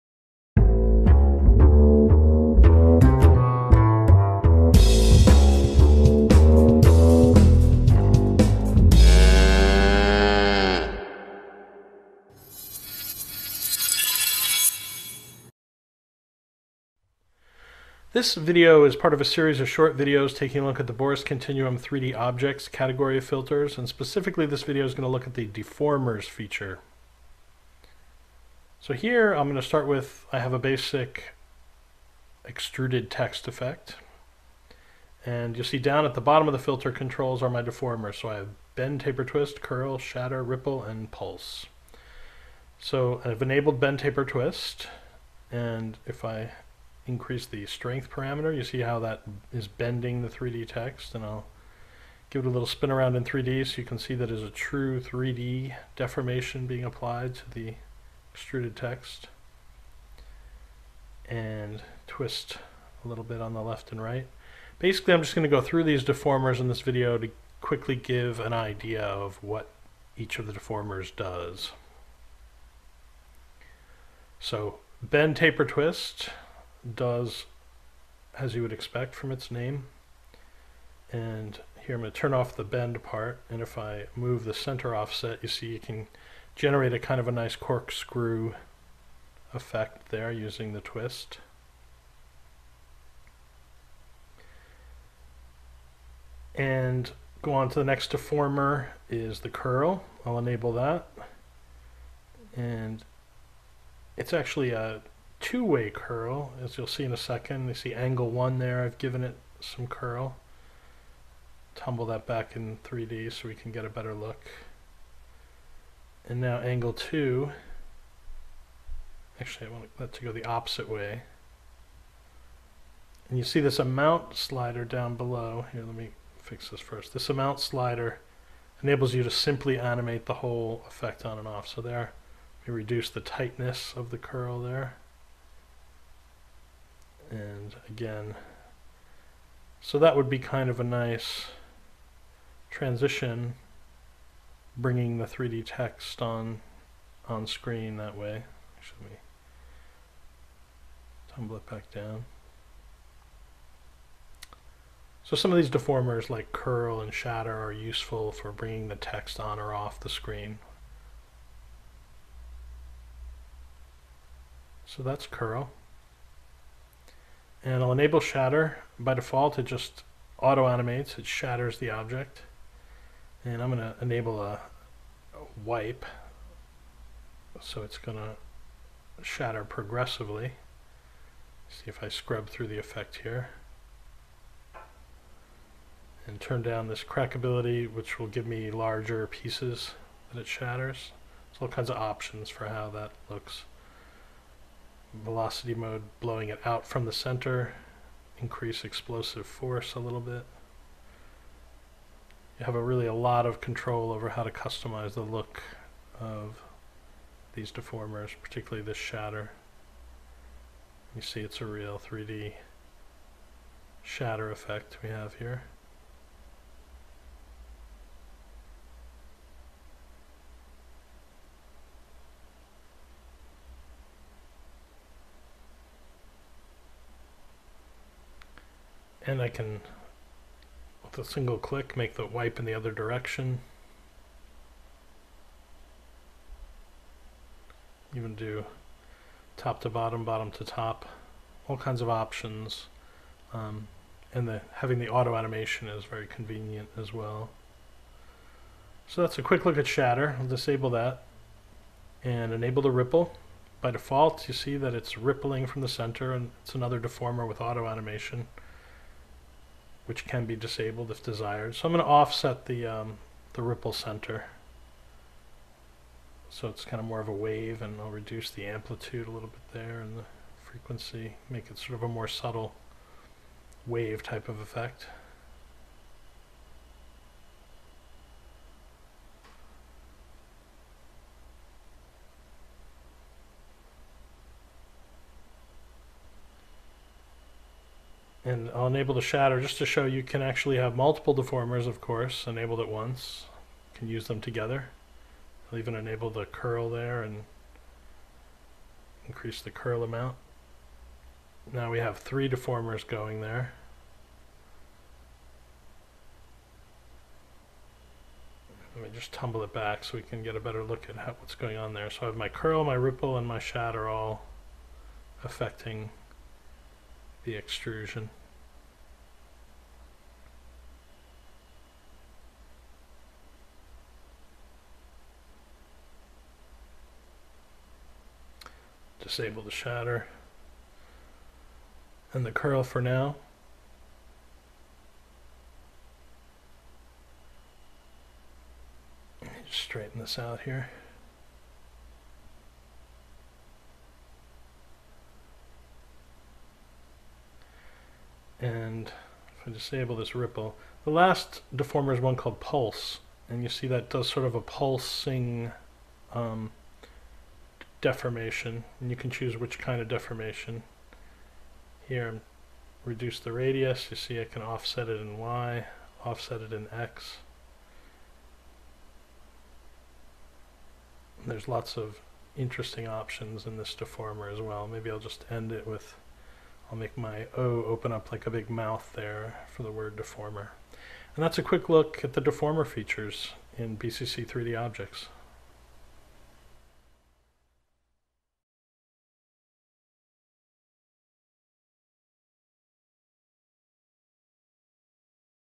Warp, Deform, and Explode 3D Extruded Text with BCC6 AE
In this Boris FX video tutorial we show you how to employ the power of vertex shaders to warp, deform and even explode 3D extruded text using Adobe After Effects and the new 3D Objects from Boris Continuum Complete 6 AE.